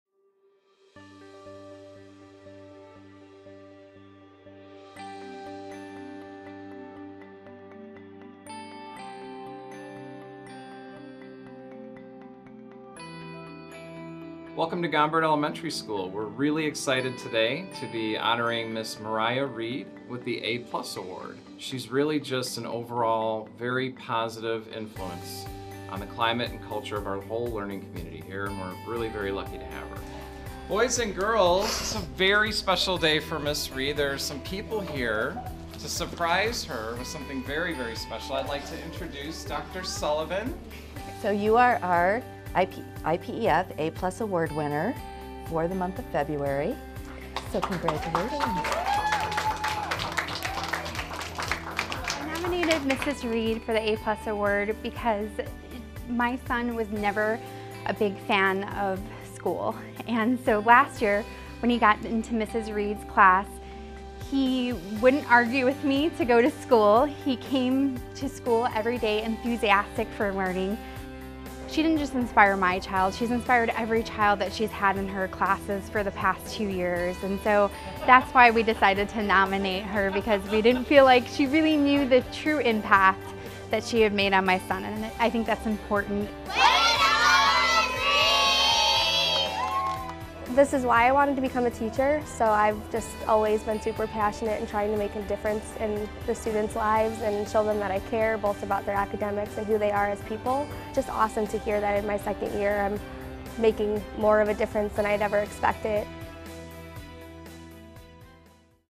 A+ Award: Mariah Reed
Mariah Reed, Gombert Elementary Teacher, is awarded the A+ Award for February 2018.